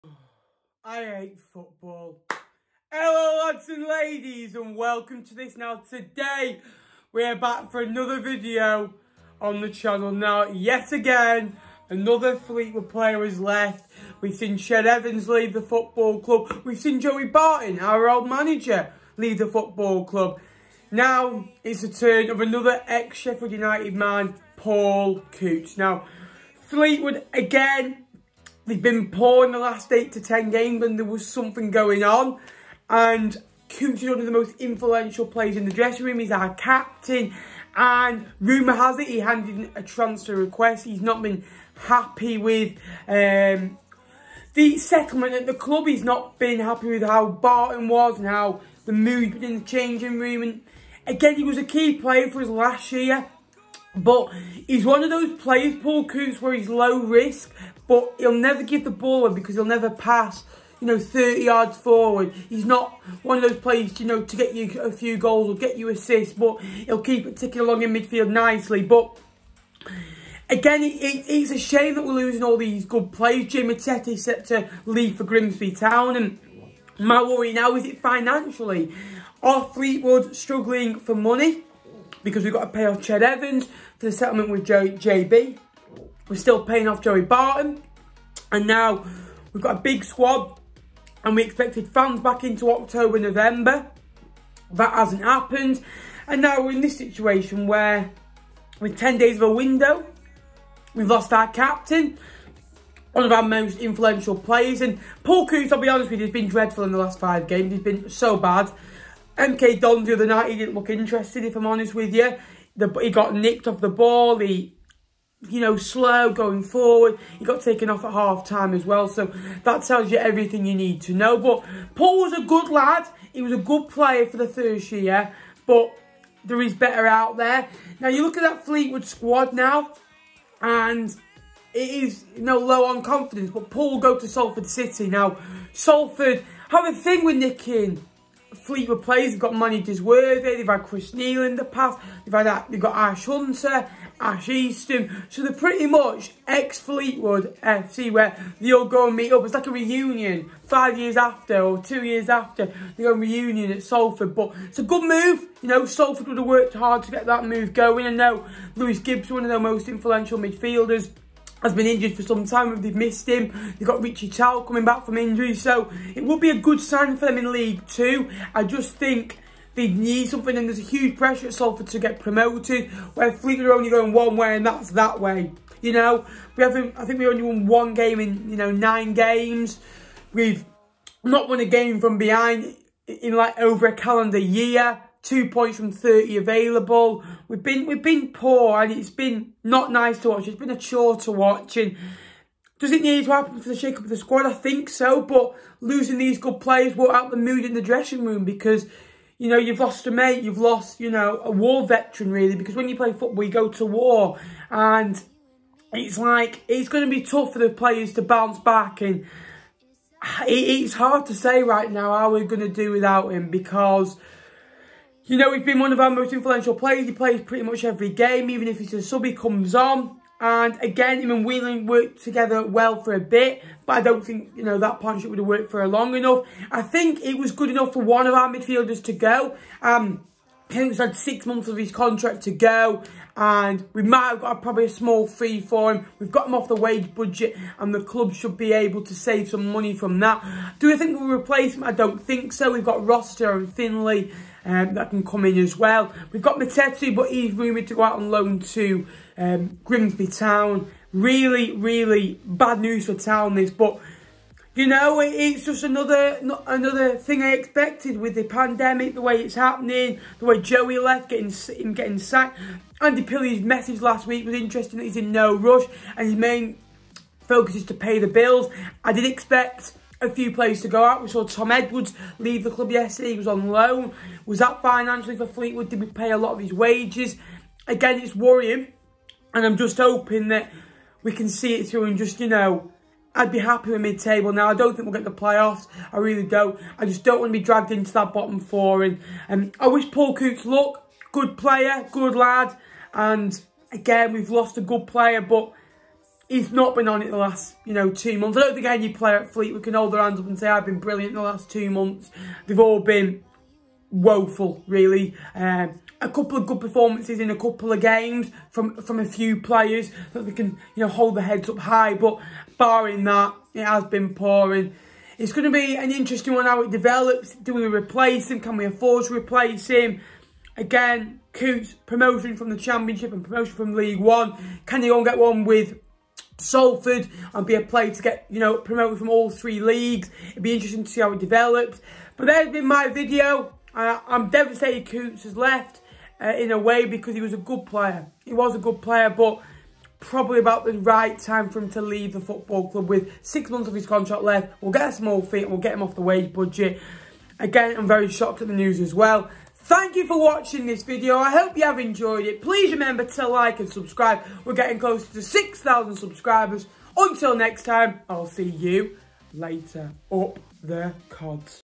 PAUL COUTTS LEAVES FLEETWOOD TOWN! | MY REACTION!
Another day. Another departure. Paul Coutts departs Poolfoot farm on a undisclosed fee to League Two side Salford City.

Here is my honest reaction to the departure..,